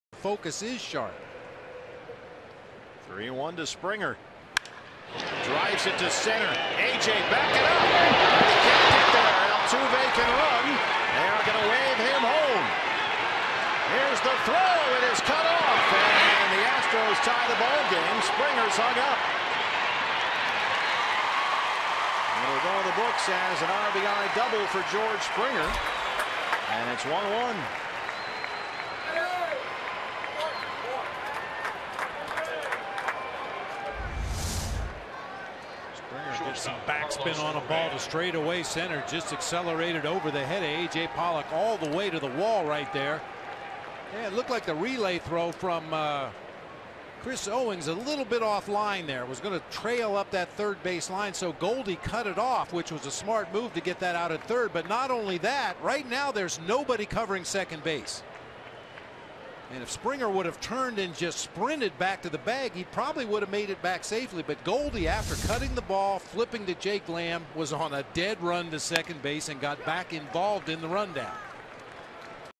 HOU@ARI: D-backs cut down Springer with rundown
10/4/15: Paul Goldschmidt cuts off a throw home and fires to third to start a rundown that eventually retires George Springer